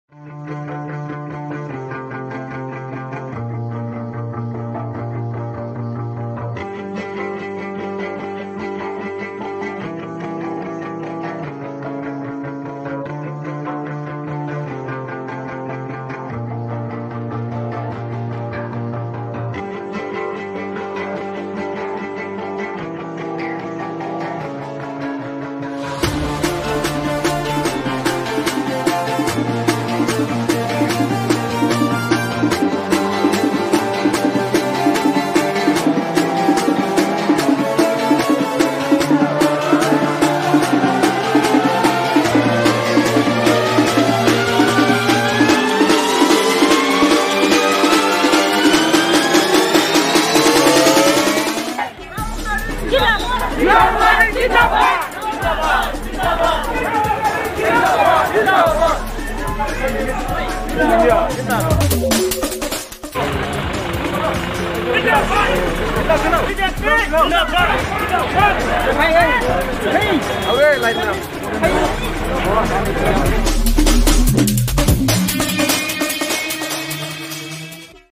High voltage BJP Campaign for NCHAC election
NCHAC Election  High Level BJP Poll Campaign to Swipe with Absolute Majority to form next council